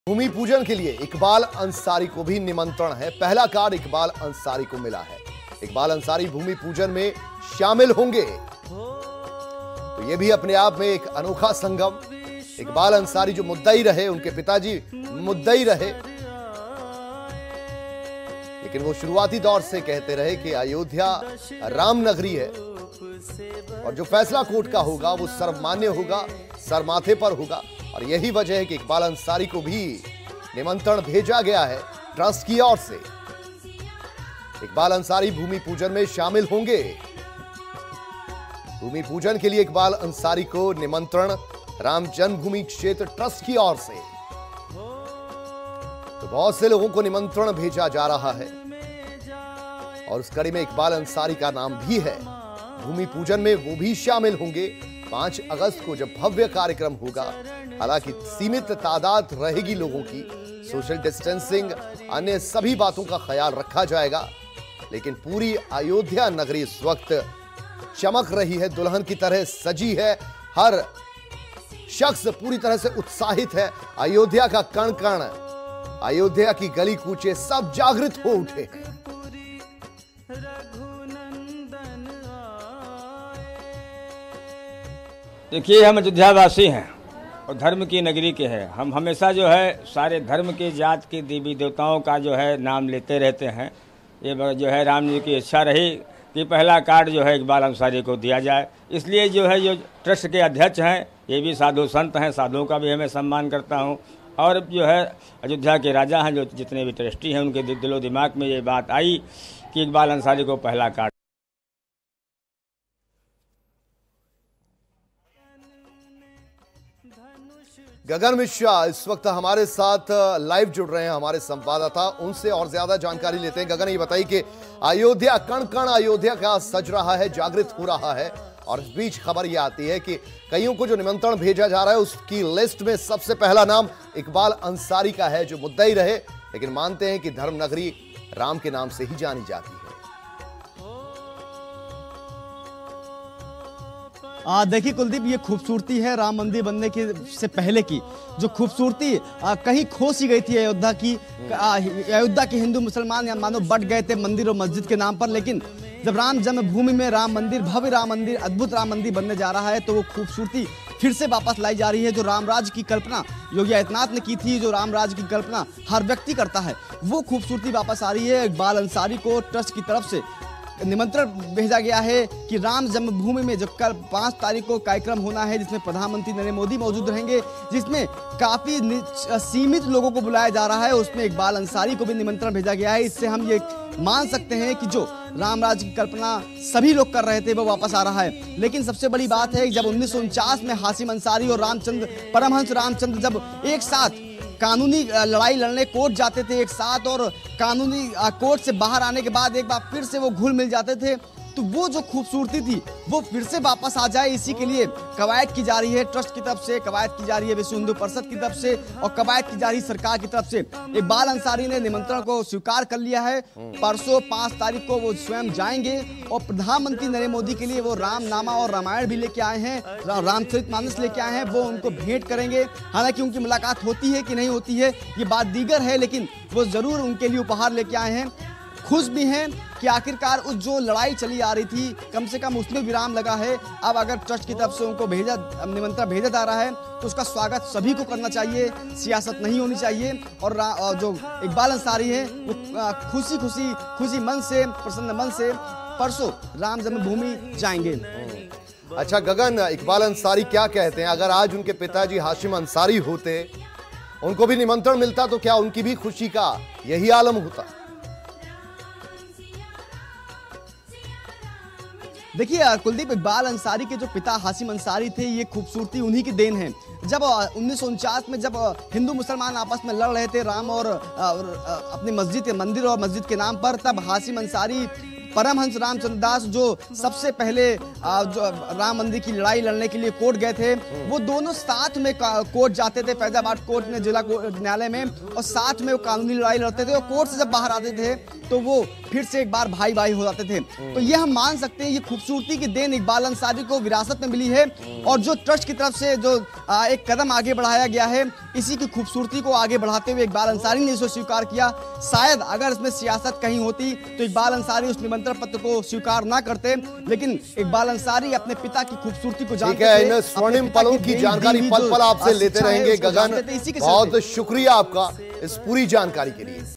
अयोध्या: भूमि पूजन के लिए इकबाल अंसारी को भेजा गया निमंत्रण का पहला कार्ड R9TV
भूमि पूजन के लिए इकबाल अंसारी को निमंत्रण
पहला कार्ड इकबाल अंसारी को मिला
इकबाल अंसारी भूमि पूजन में शामिल होंगे